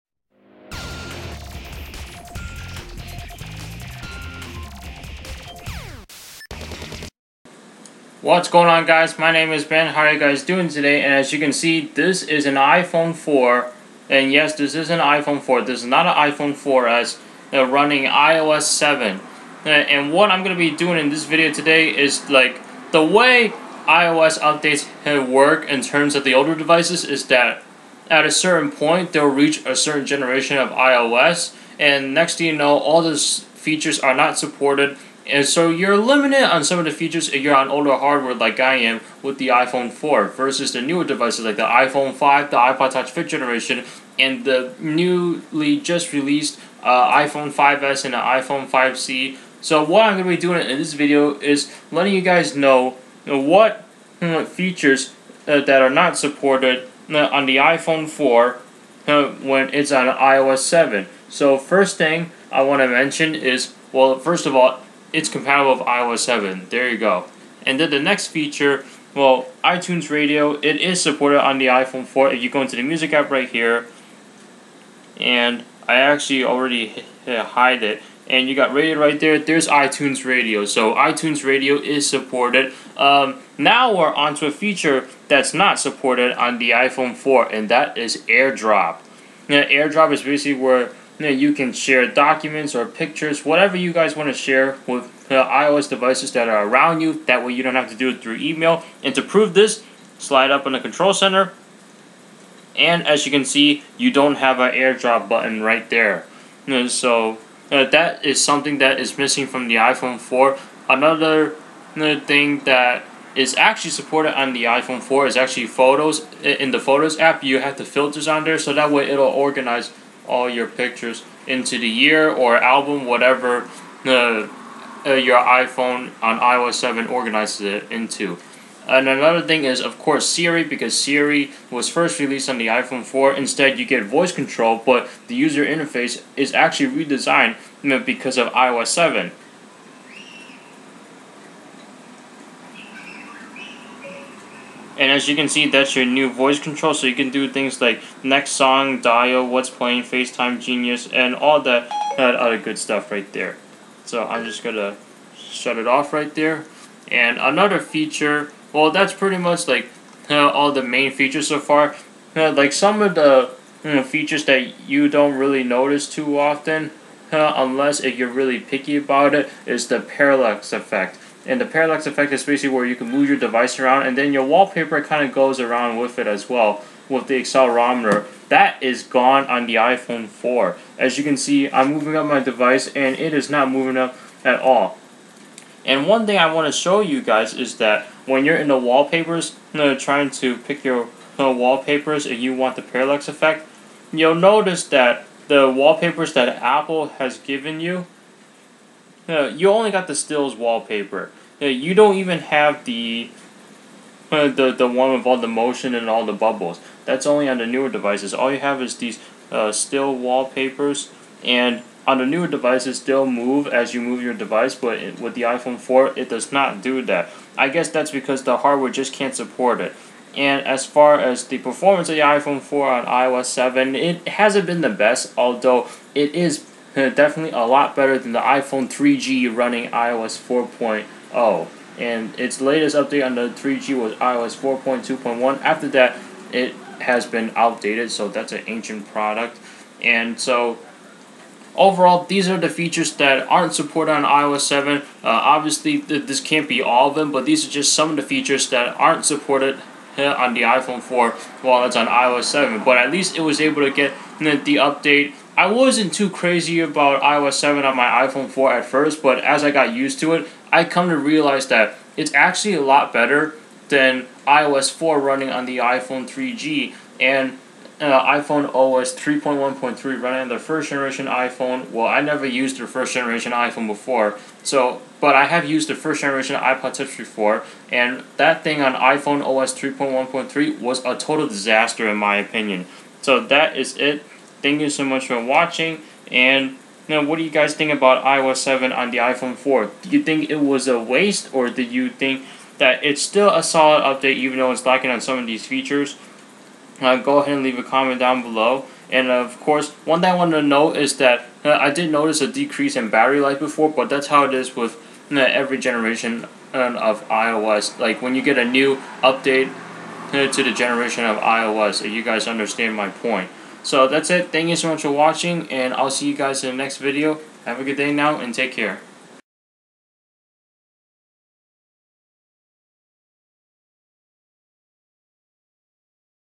iOS 7 Features Not Supported on iPhone 4
So iOS 7 is supported on the iPhone 4 so that way it is not falling behind and not be outdated so soon. However, there are some features in iOS 7 that are not supported on the iPhone 4. If you are an iPhone 4 user like I am, do you know whaat features are not supported on the iPhone 4 unlike the newer devices? Stick around and find out as I show you some of the features that are not supported on the iPhone 4.